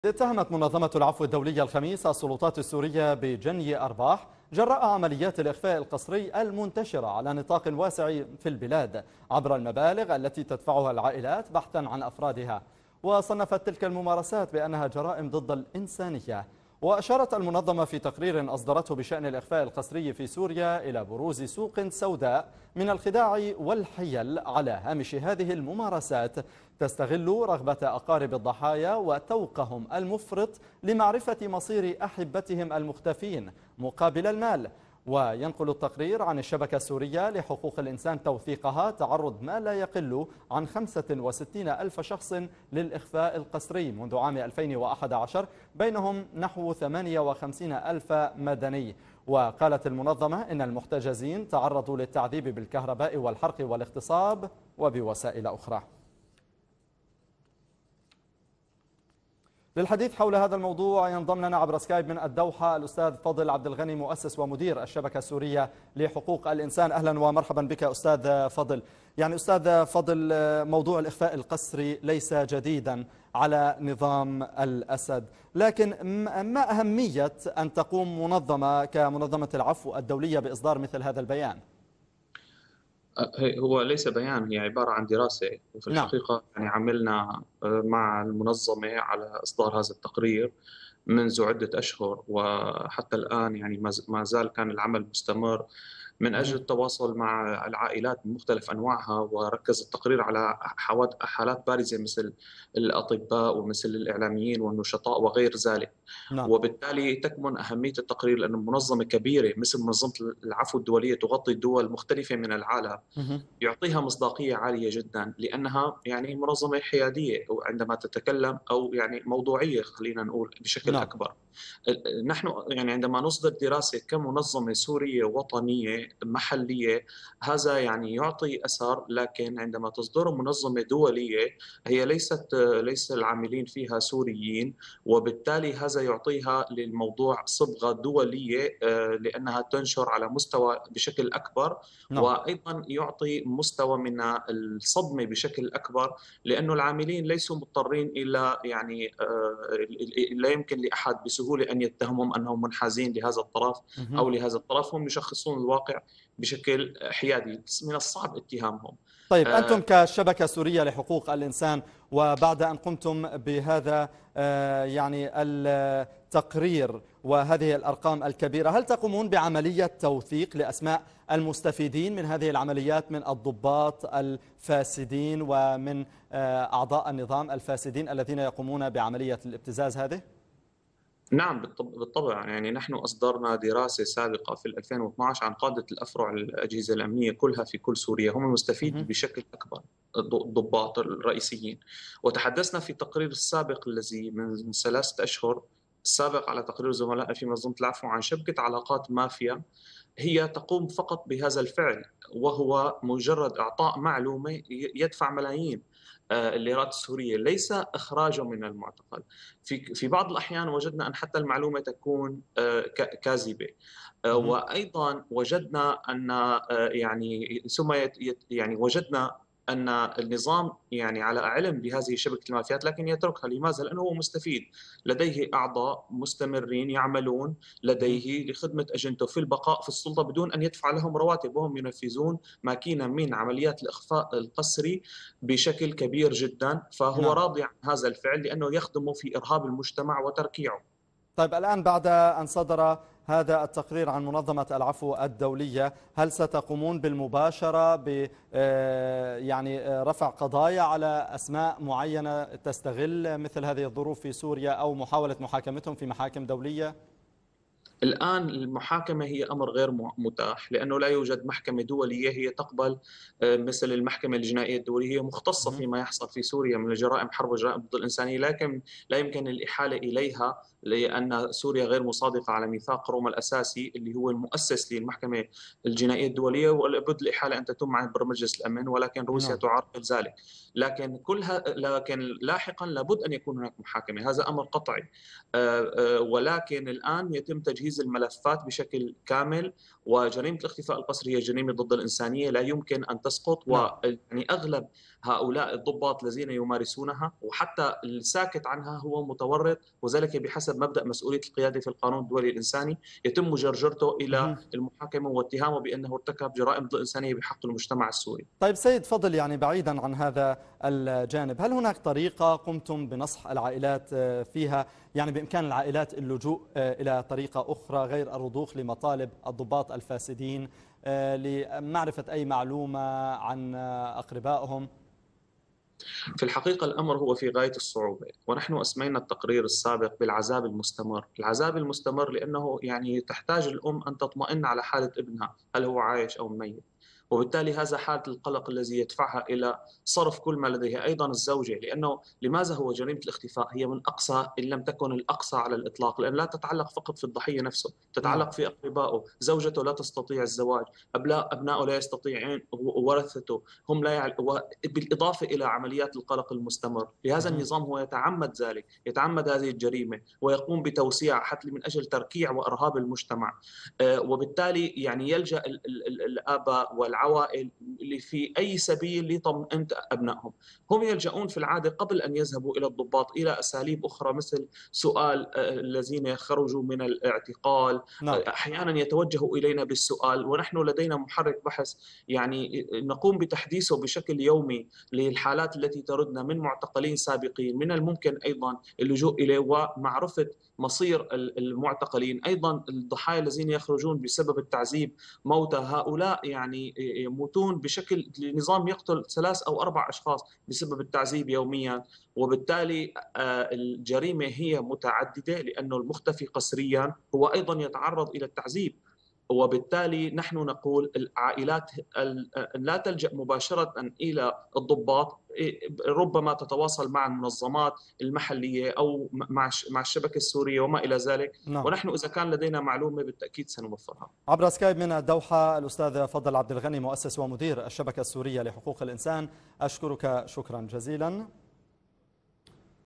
العفو الدولية: 65 ألف شخصا اختفوا قسرا في سوريا
شاهد آيضا:
قيادي بعثي يفضح تاريخ آل الأسد!؟
اتهمت منظمة العفو الدولية الخميس السلطات السورية بجني أرباح جراء عمليات الإخفاء القسري المنتشرة على نطاق واسع في البلاد، عبر المبالغ التي تدفعها العائلات بحثا عن أفرادها، وصنفت تلك الممارسات بانها جرائم ضد الإنسانية.

وأشارت المنظمة في تقرير أصدرته بشأن الإخفاء القسري في سوريا إلى بروز سوق سوداء من الخداع والحيلة على هامش هذه الممارسات، تستغل رغبة أقارب الضحايا وتوقهم المفرط لمعرفة مصير أحبتهم المختفين مقابل حفنة من المال.

ووصل عدد حالات الإخفاء القسري بسوريا وفق منظمة العفو إلى مستويات مروعة، وينقل التقرير عن الشبكة السورية لحقوق الإنسان توثيقها "تعرض ما لا يقل عن 65 ألف شخص للإخفاء القسري منذ العام 2011 بينهم نحو 58 ألف مدني.
وقالت العفو إن المحتجزين تعرضوا للتعذيب بالكهرباء والحرق والاغتصاب ووسائل أخرى.